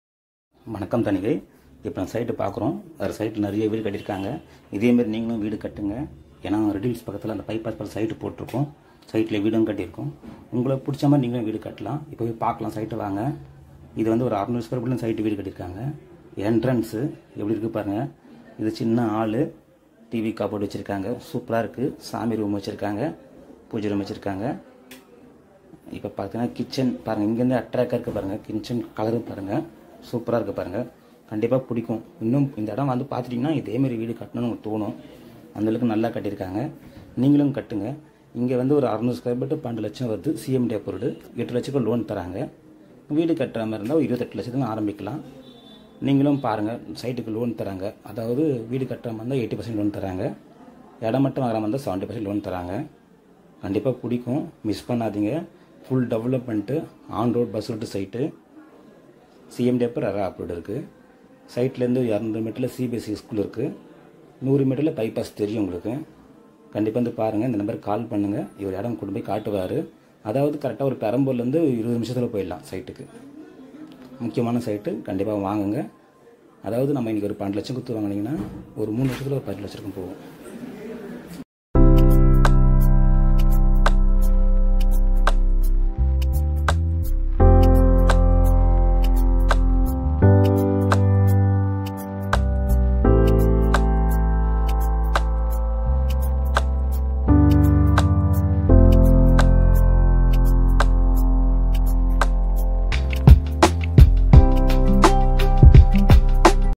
REDHILLS ORR Road View Plot !!! | Thanigai Estate | Gomathi Amman Nagar | CMDA RERA APPROVED
🌻🙏 Just 100 mtrs from 400 ft Outer Ring Road.

 👍CMDA APPROVED
 👍Ready to Construction Plots Already Have houses in our Site
👍EB & Water facilities

 👍Rs.1999 / - per square feet
 👍 Advance Rs.  100,000 / -

 👍600 sqft - Rs.11,99 400/ -
 👍Bank has credit facility.
 There is a 6 month installment plan.

 🏕️Bond Registration + Personal + EC is completely free if you pay the full amount within 30 days.

 🏕️Bond registration is free only if the full amount is paid within 90 days.
n
 🏕️The bond registration cost is theirs if the full amount is paid within 180 days

 🌷Note: -
 Deed registration is free if half of the amount due for the land is paid immediately.

  🚗Free land viewing facility on all days.